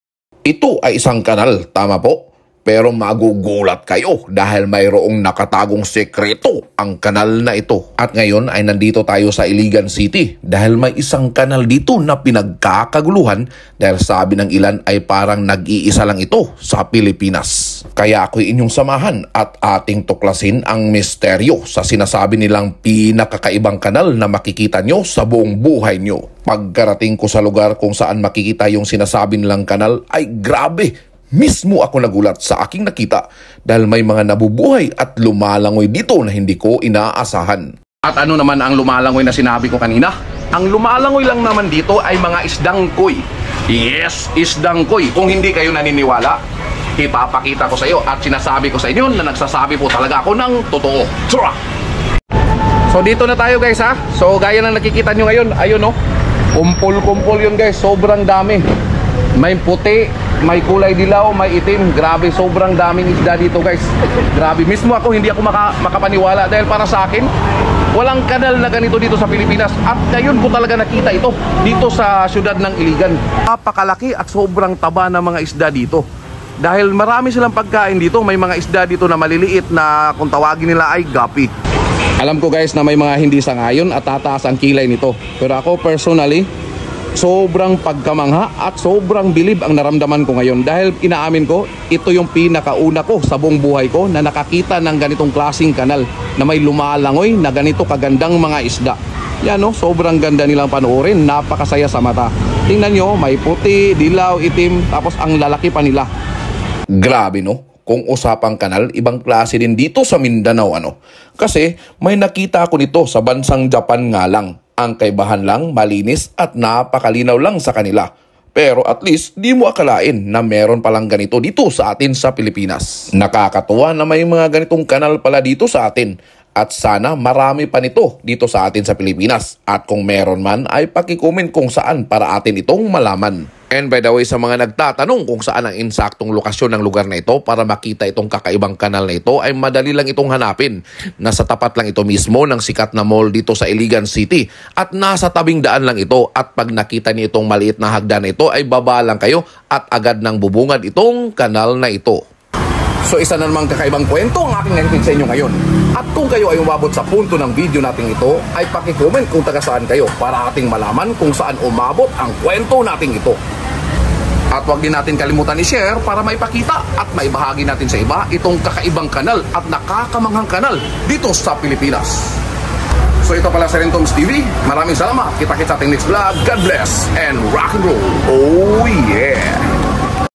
KAKAIBANG KANAL SA BOUNG PILIPINAS
Ipapakita ko sa inyo ang pinakakaibang kanal sa boung Pilipinas na makikita lamang sa syudad nang Iligan..Kakaiba ito dahil may mga nabubuhay at lumalangoy dito na hindi mo kalimitan nakikita sa isang kanal